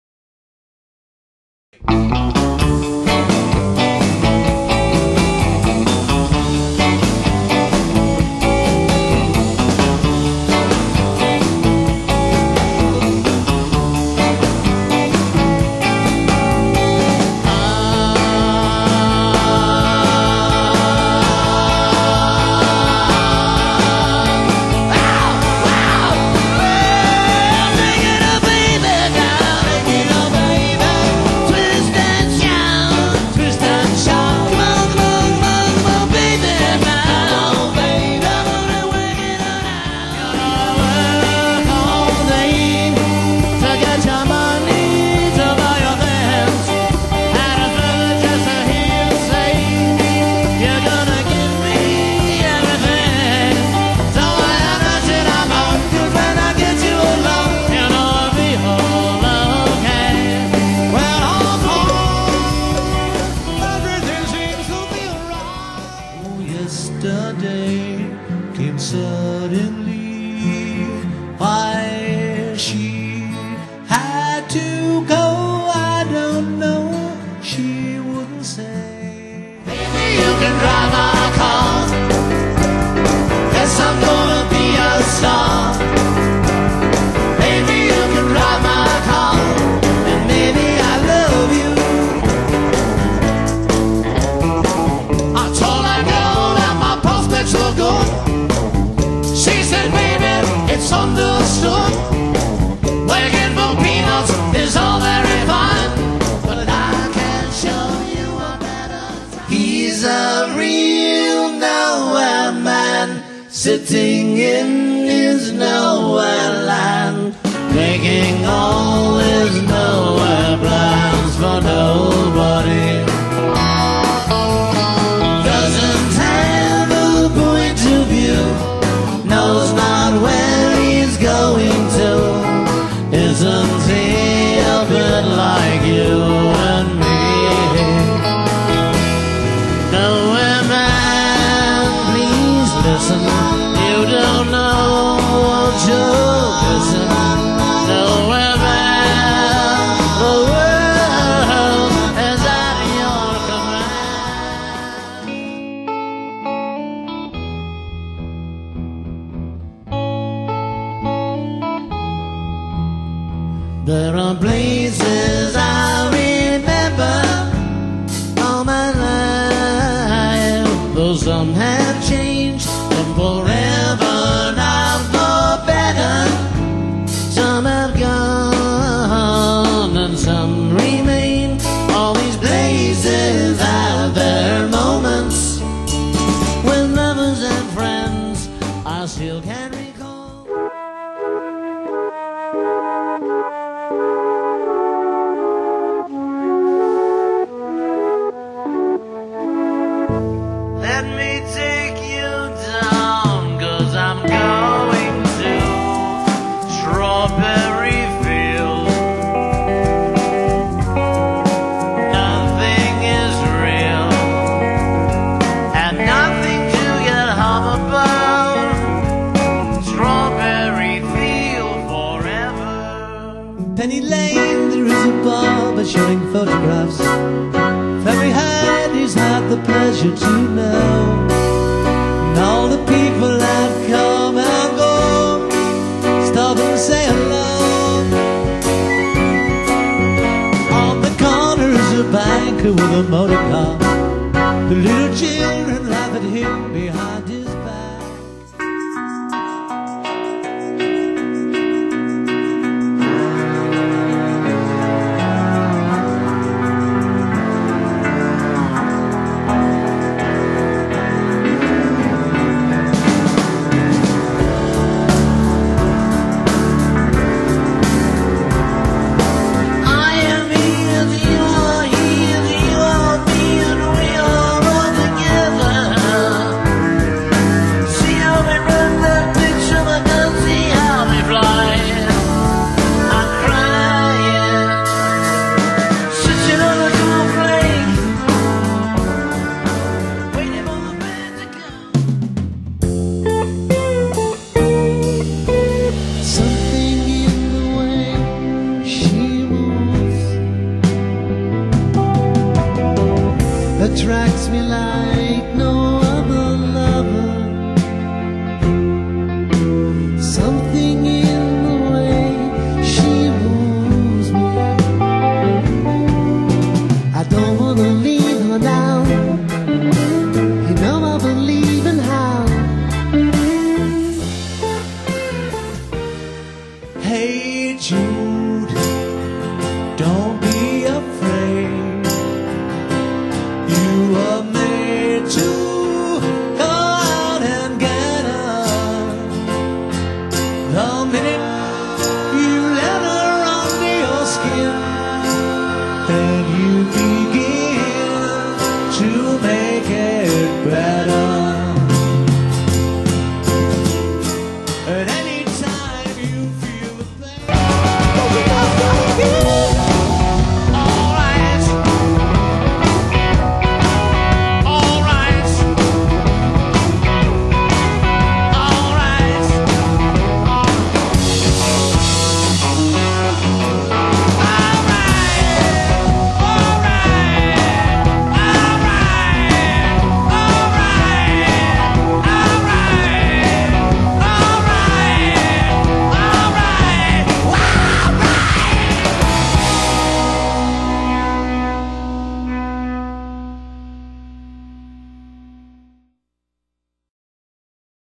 The Cast of Beatlemania
This video was recorded live in a studio with no tracks or overdubs. Yes they are really singing! Its all real. Enjoy!